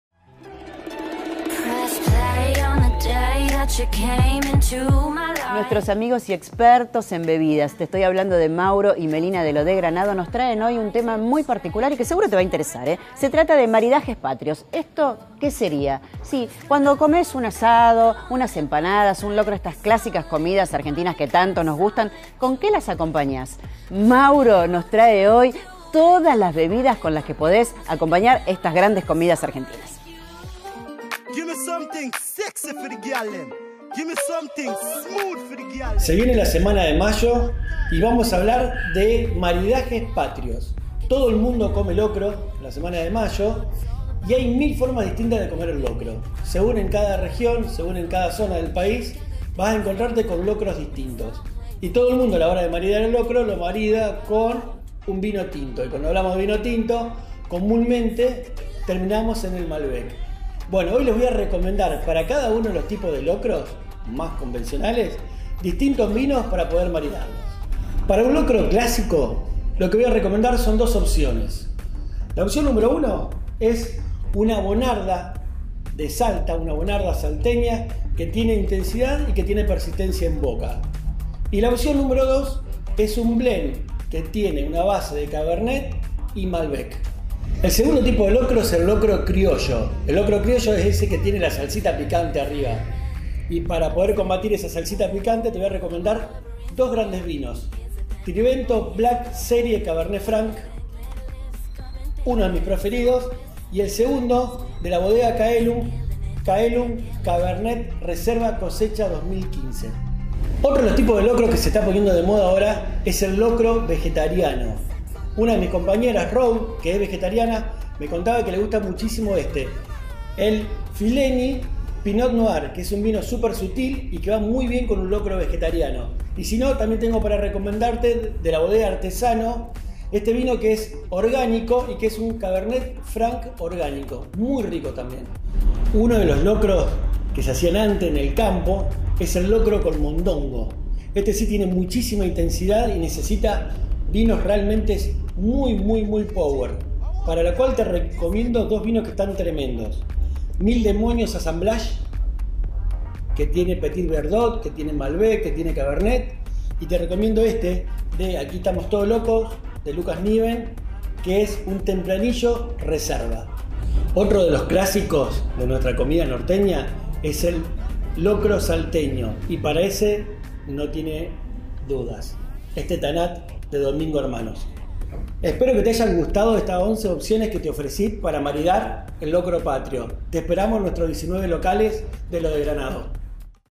Nuestros recomendados patrios para la semana de mayo
Participamos del Social TV TELEFE Rosario recomendando vinos para maridar el locro patrio.